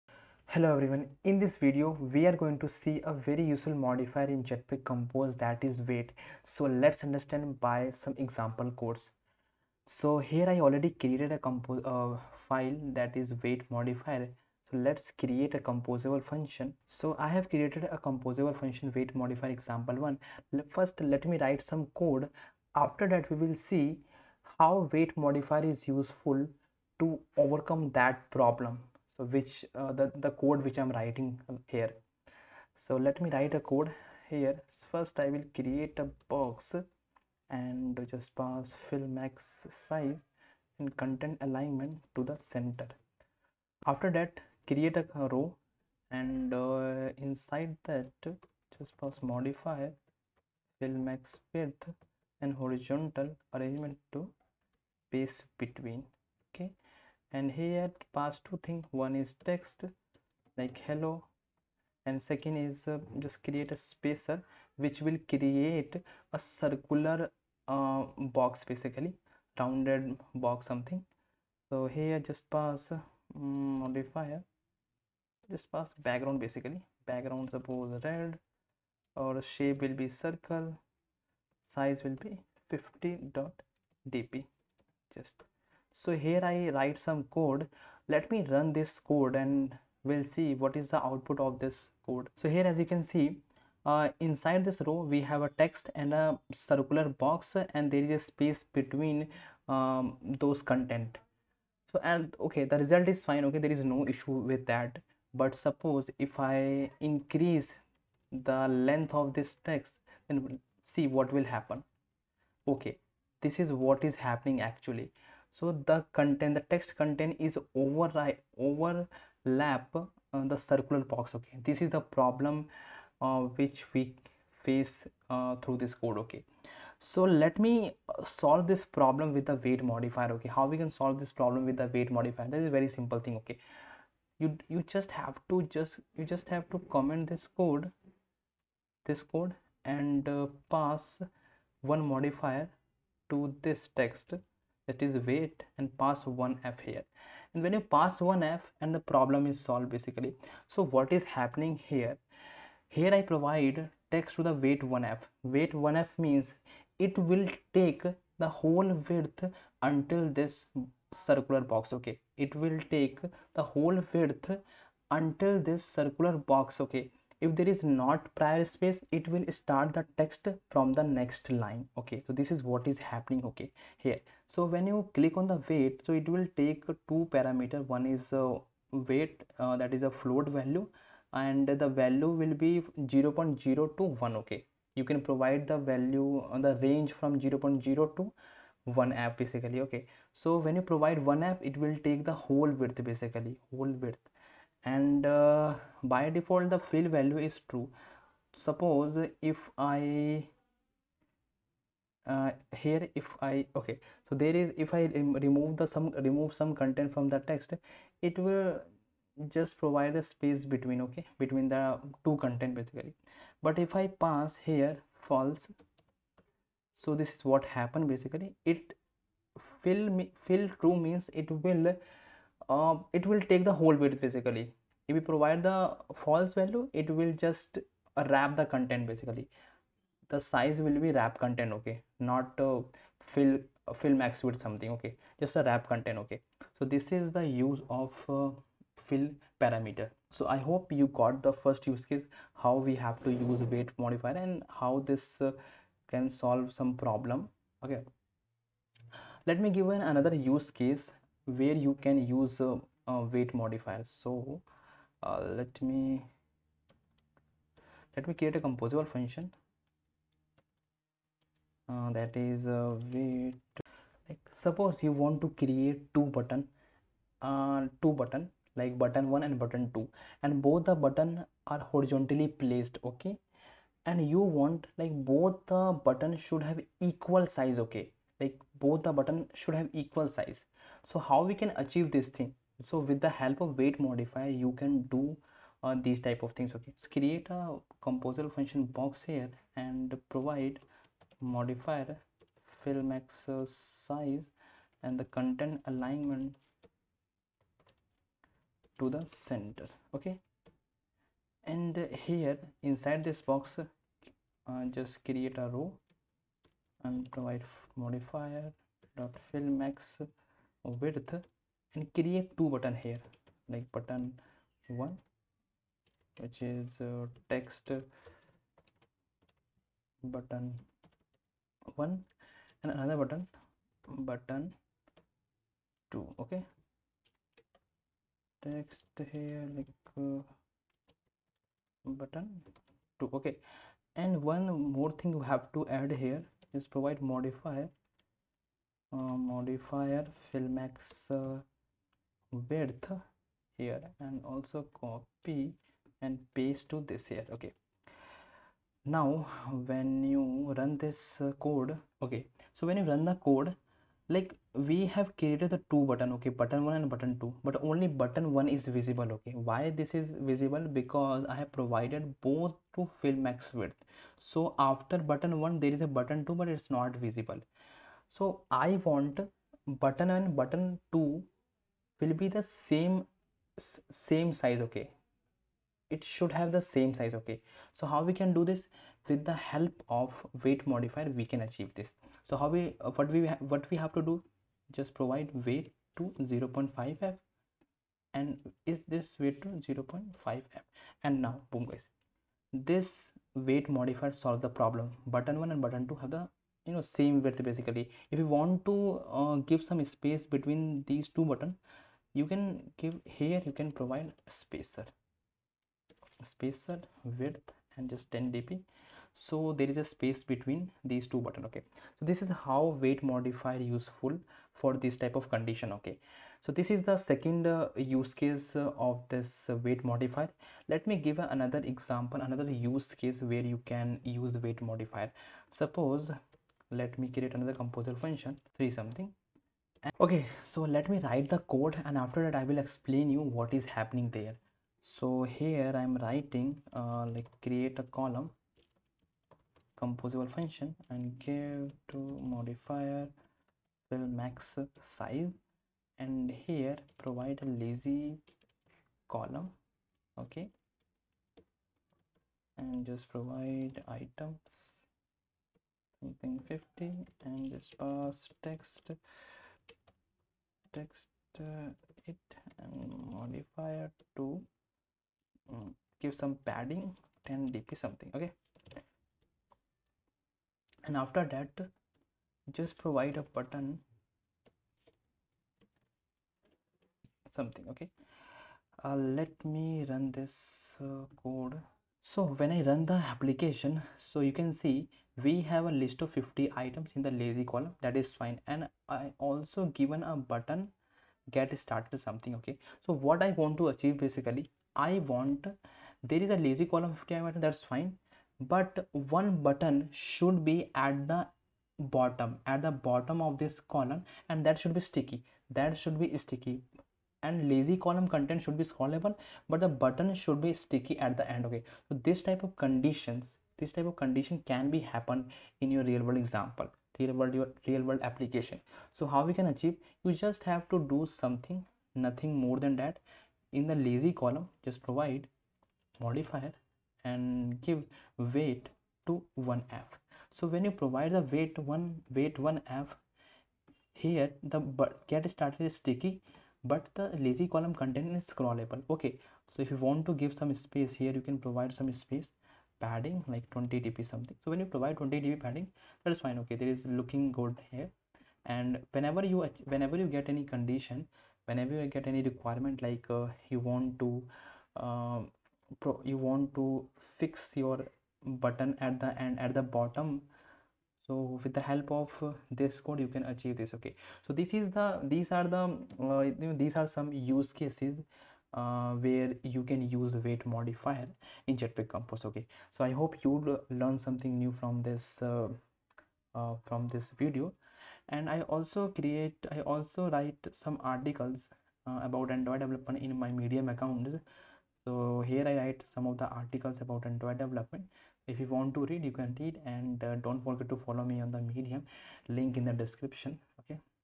Weight Modifier in Jetpack compose | Bootcamp #17
So in this video we will learn about weight modifier in jetpack compose.

Keep Leaning happy coding❤️ 🤩